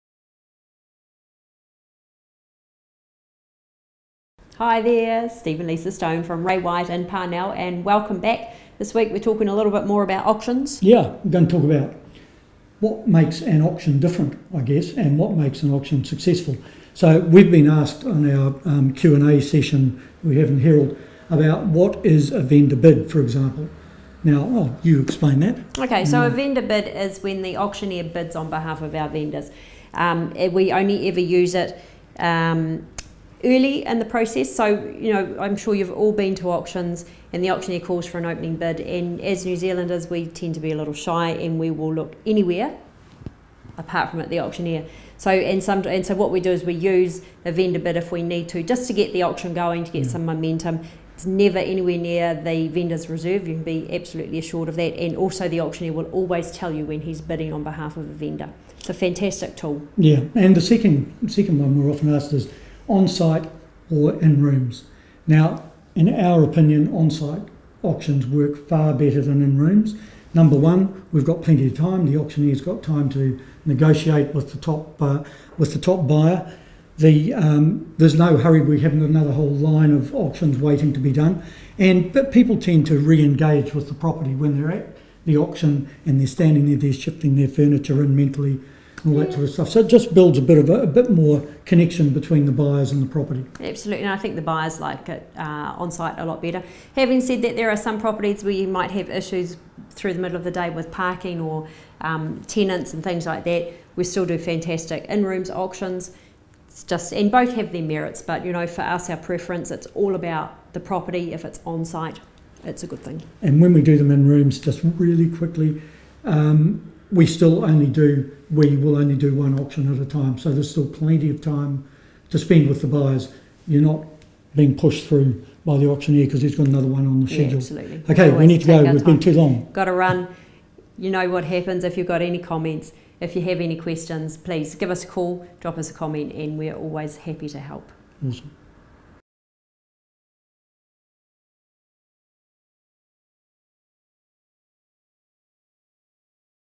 The Stones - Selling at Auction Part 2
DOES AN AUCTION SOUND INTIMIDATING? IT IS THE BEST WAY TO SELL!

We cover the process of selling at auction and how it is the best way to sell. Before the auction there is marketing and open homes. The number one benefit of selling at auction is that it is an easy and quick process. We also explain what a vendor bid is and the pros and cons of onsite auctions.